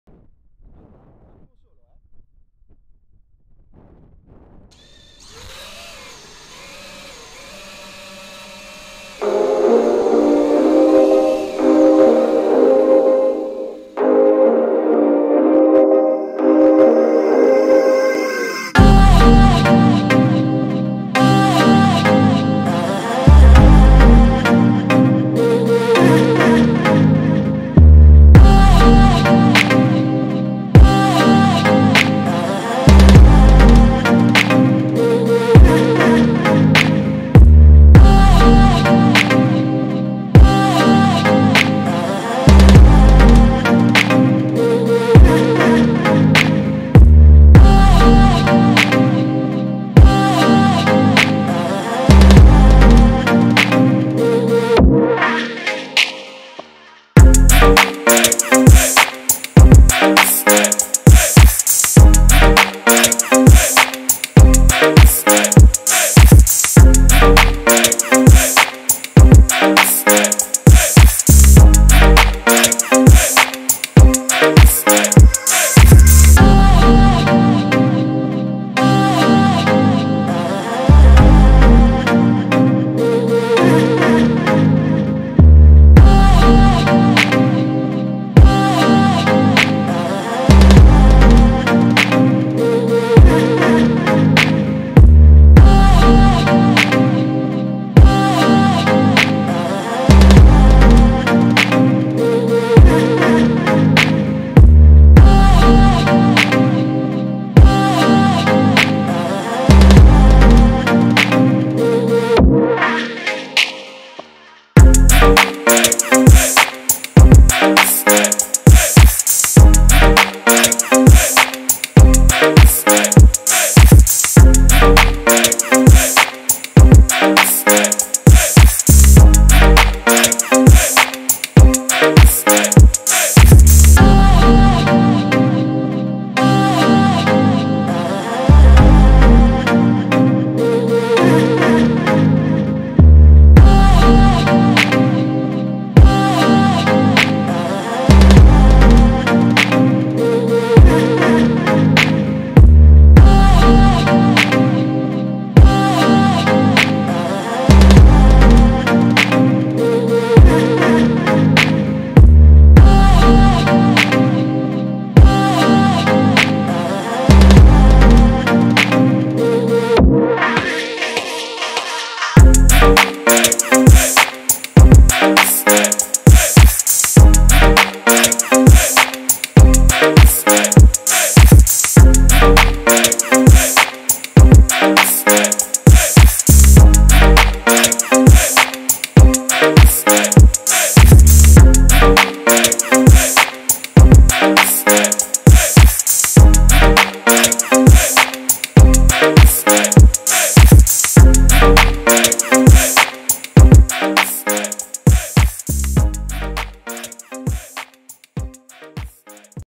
PLANE CHASE
Il giorno della Befana sono stato invitato da Emanuele al campo volo di Villanova di Marsciano. Una splendida giornata con vento abbastanza forte. Ancora alla ricerca del tuning perfetto col mio nuovo drone ho avuto l'opportunità di "inseguire" un'ala fissa. Ringrazio per l'ospitalità e quando volete fare altre riprese aeree fate un fischio!
The day of the Epiphany I was invited from Emanuele at the Flying site of Marsciano in Villanova. It was a beautiful day with fairly strong wind. As I was searching for the perfect tune with my new drone I had the opportunity to "chase" a fixed wing. I would like to thank the local club for the hospitality and whenever you need some aerial shots give me a bell!